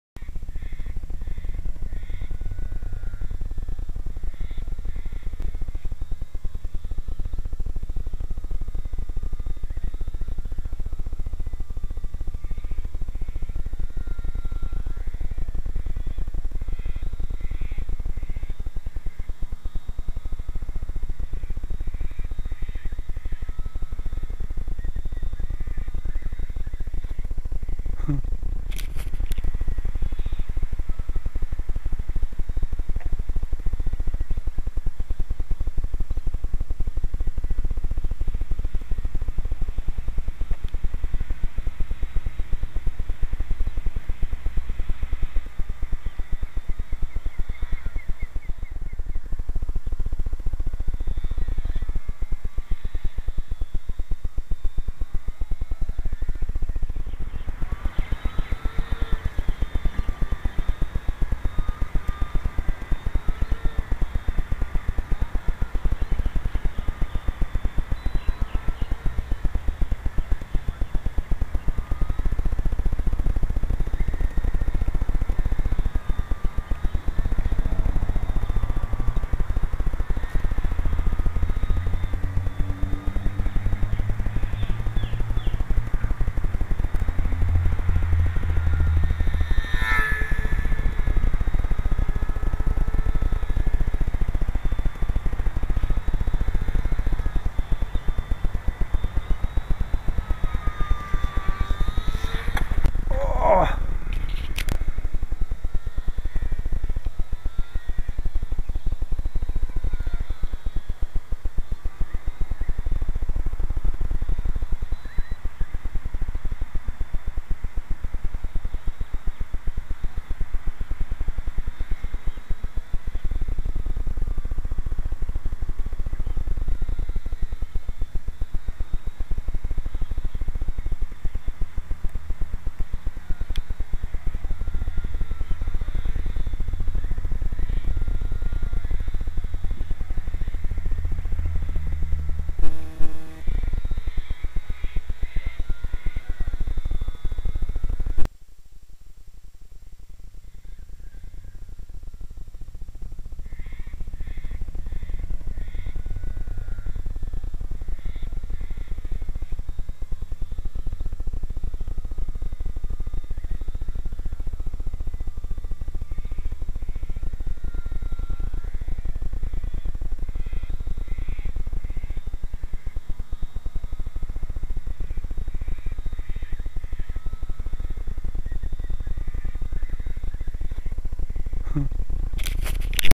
HZ CHAMP inverted flying
This was my attempts at flying the Hobbyzone Champ inverted. Its alot harder then it looks. I think it helps if theres a slight breeze.